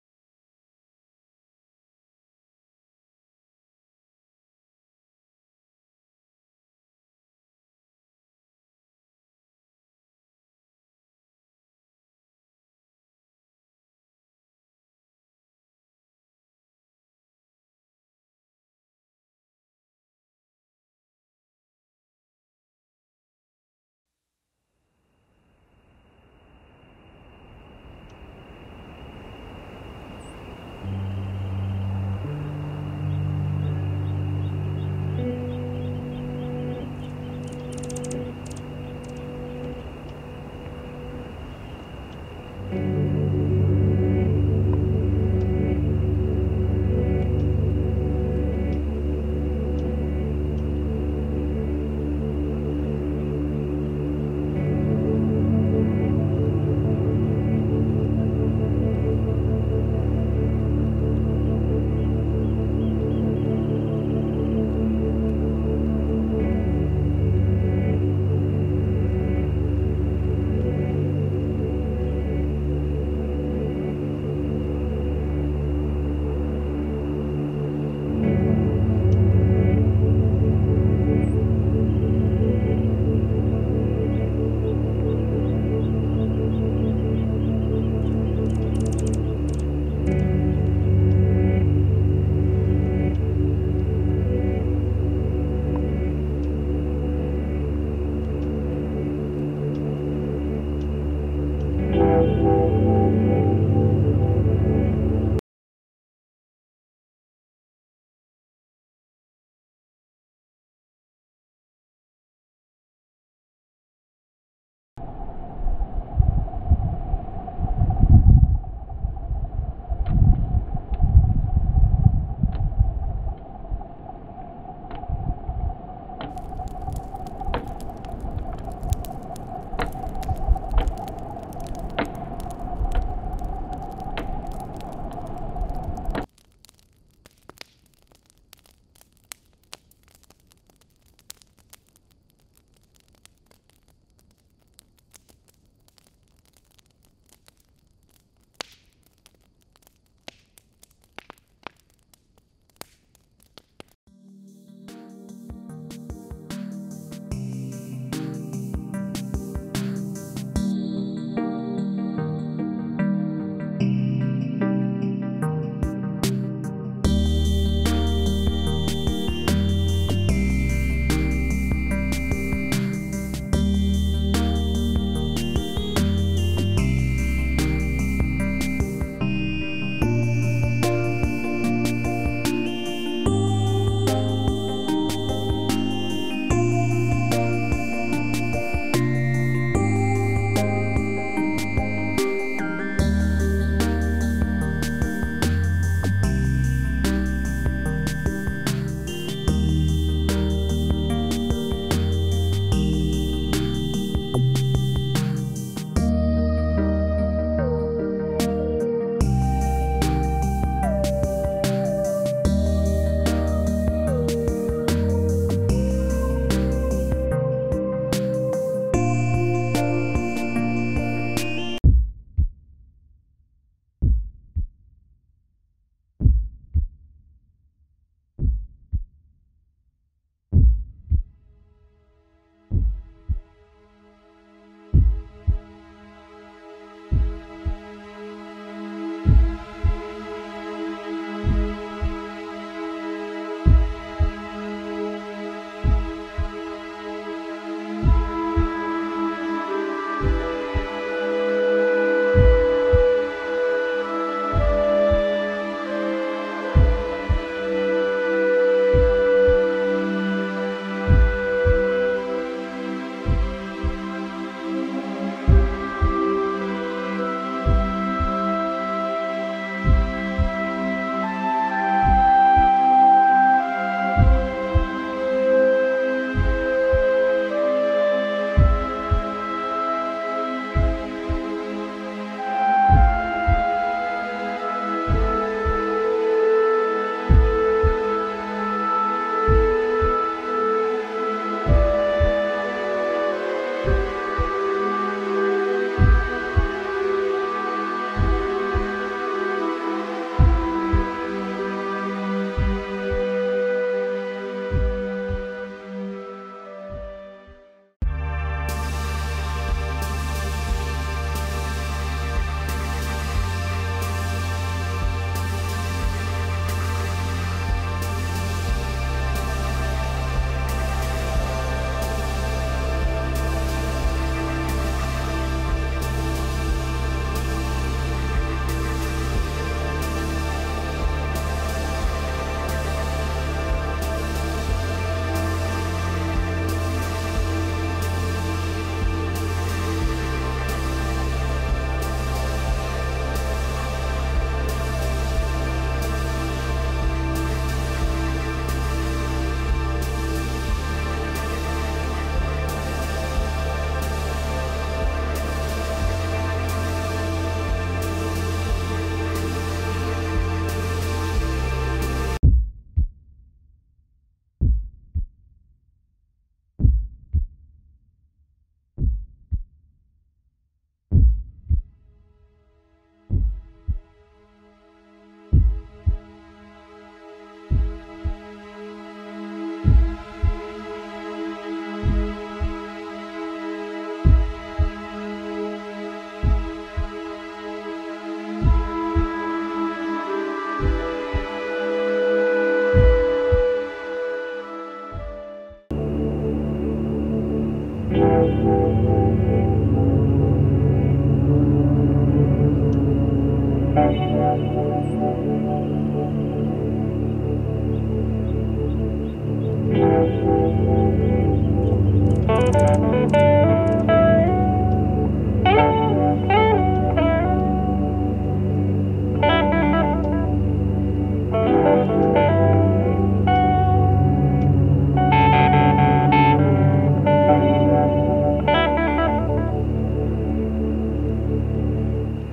Iron Plow Painting
Just painting a happy little iron plow to calm my inner demons. Hope you like it.